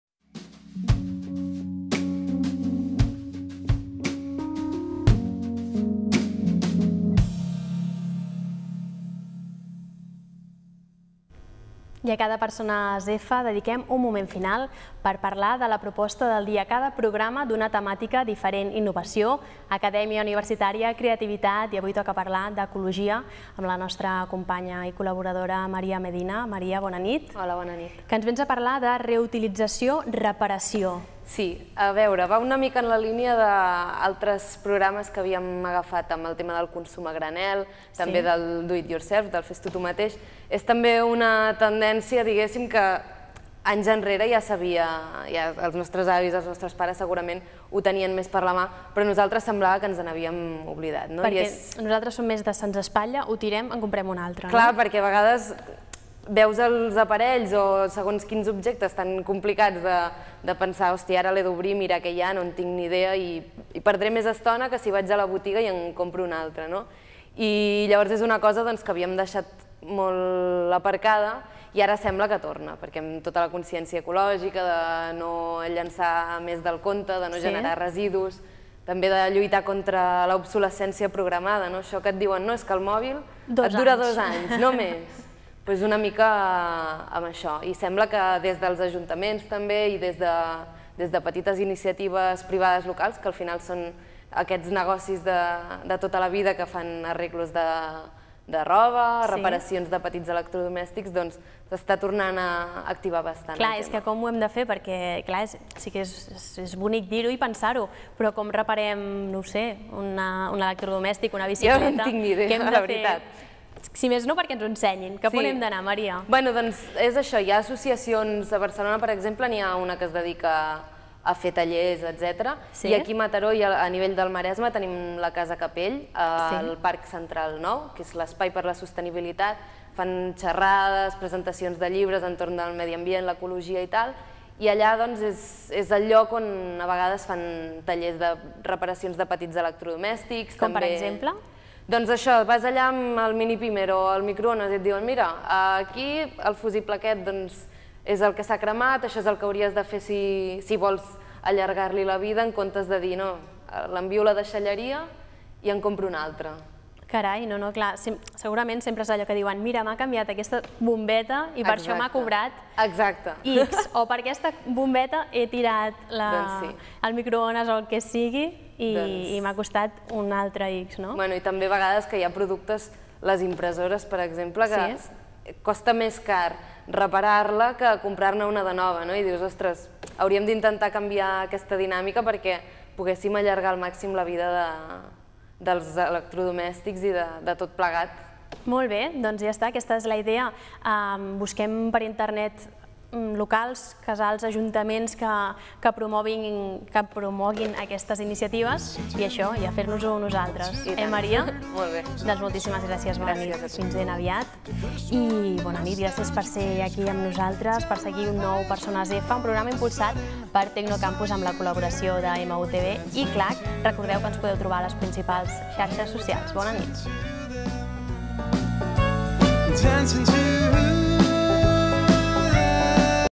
La Maria Medina ens parla d'ecologia a través de la reutilització i reparació
Degut a la consciència ecològica, actualment i des dels ajuntaments es torna a activar la idea de la reparació/reutilització de coses velles i materials reciclats per a nous usos. A Mataró en trobem la Casa Capell.